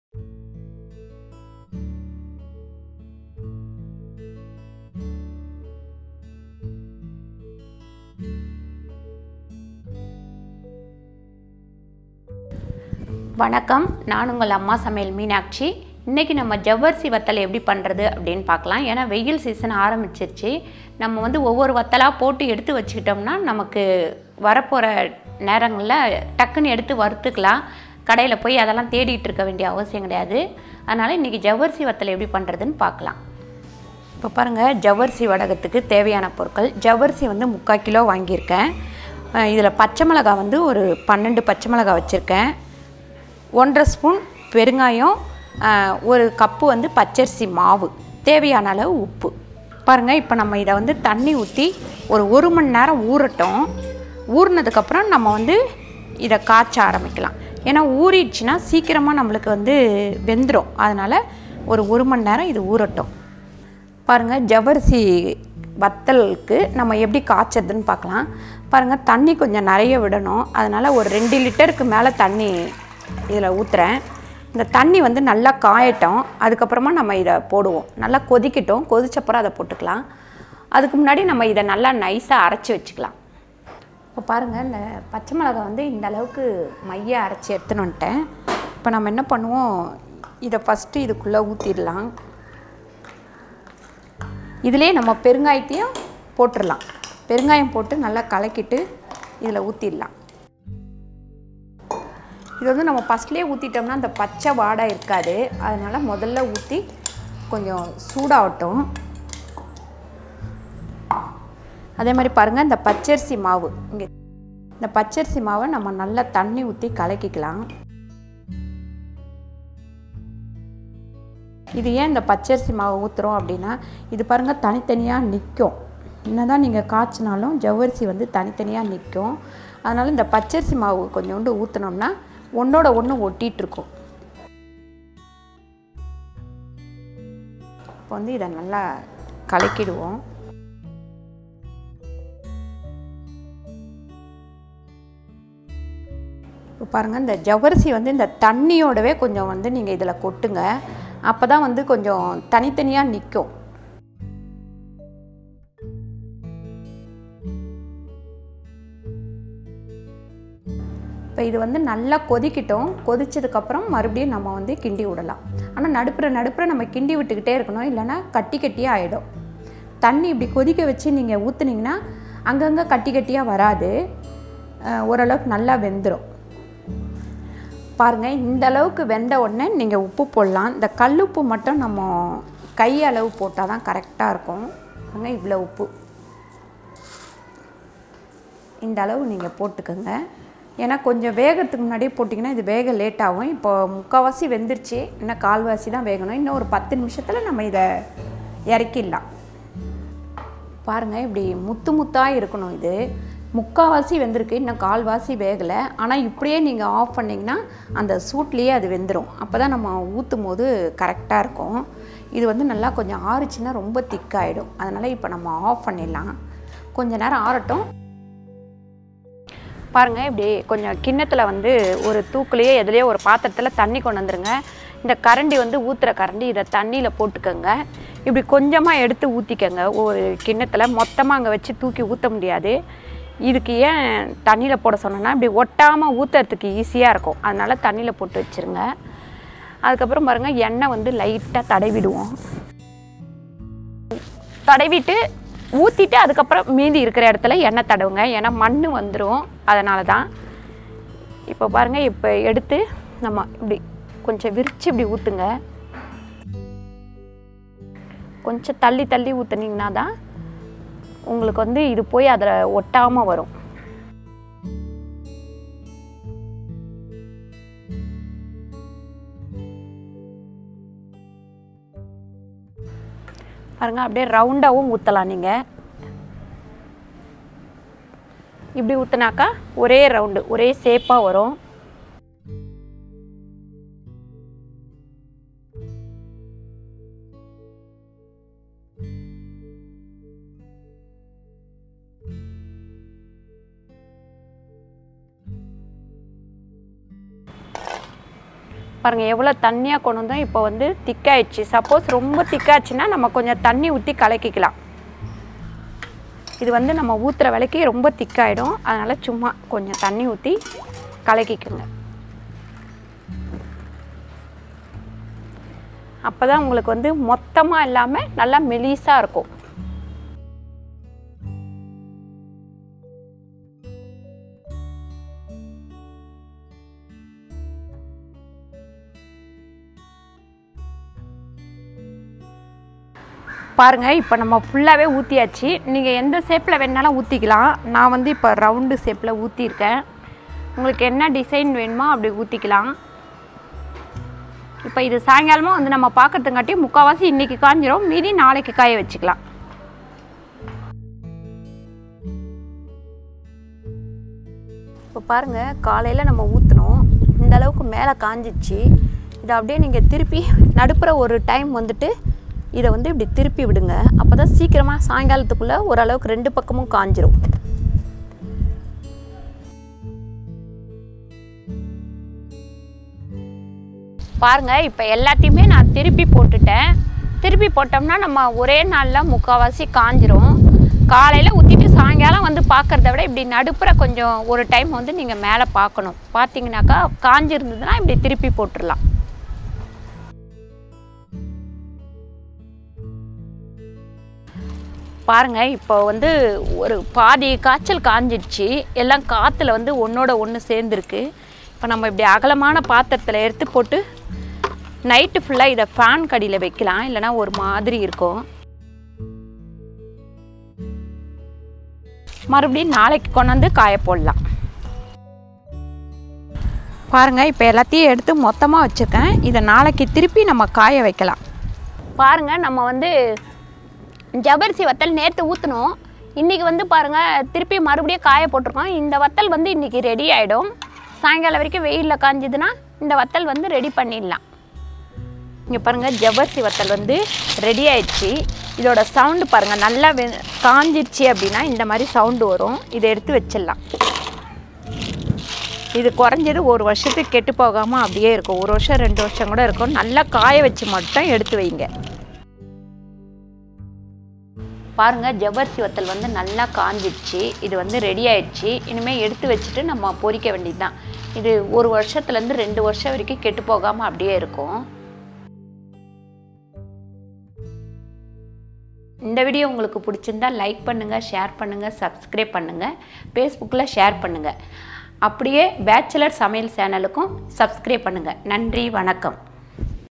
Javvarisi vadam Recipe | Sago vathal Recipe | Sago Fryums
Hi Viewers,Another vadagam or vadam for summer
special.Javarusi vadagam will come out crispy and tasty.

    Ingredients

1.sabudhana/sago 3/4kg
2.Green chili 12nos
3.Asafoetida powder 1 & 1/2spoon
4.Raw rice flour 1cup
5.Salt Required amount